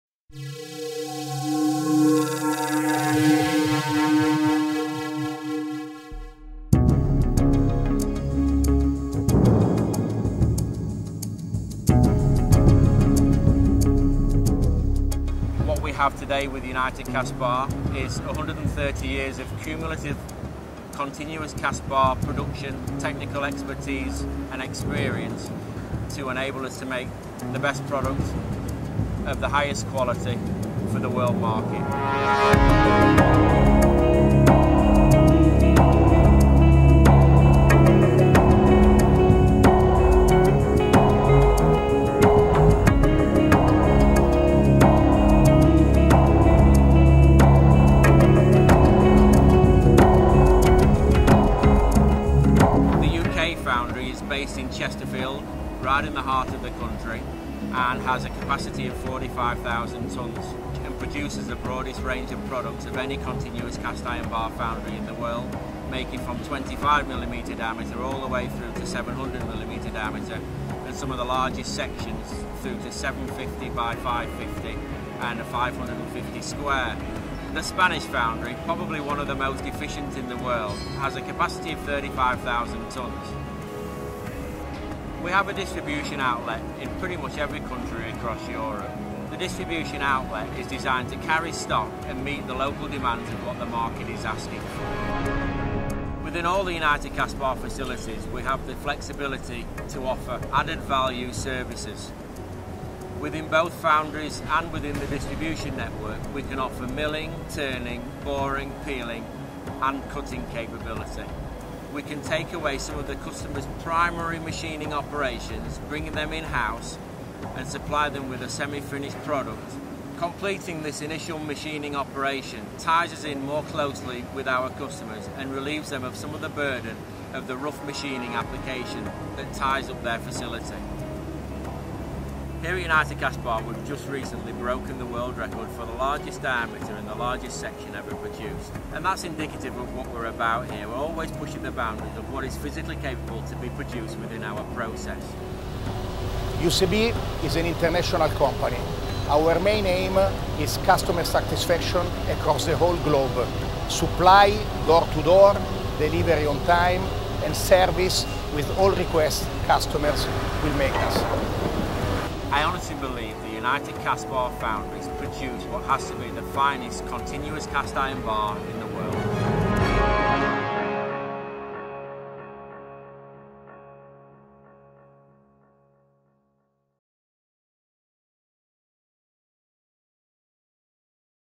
Corporate Video - Short Version
United Cast Bar Limited. was formed in February 1998 by the merger of three of Europe's most prestigious continuous cast iron bar foundries: Eurocast Bar Limited (UK), Starkey's Technicast Limited (UK) and Cast Profil SA (Spain), together with their associated stockists and service centres.

Bringing together over 120 years experience into one group of companies, United Cast Bar developed a synergy for technical and manufacturing procedures, quality systems, control parameters, commercial rationalization, and supply logistics. From this base United Cast Bar has created the Unibar brand of products.

This business process resulted in the formulation of United Cast Bar's group wide 'Procedure For Best Practice', accompanied by a structure of contained costs which reinforce and guarantee our competitive position in all markets.

United Cast Bar strengthened and extended its sales structure and logistics network by providing a manufacturing base to produce over 80,000 tonnes of Unibar, with over 20,000 tonnes permanently held as standard stock in its foundries and distributors, throughout Europe, North America and the Far East.

Our aim with Unibar is to establish standards of quality, consistency and customer service that exceed current market demands, and to provide the best technical and commercial support.